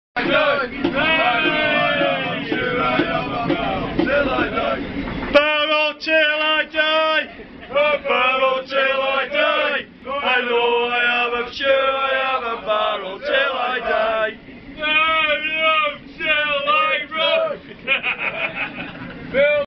Barrow fans on the train
Barrow fans on the train back from wukkie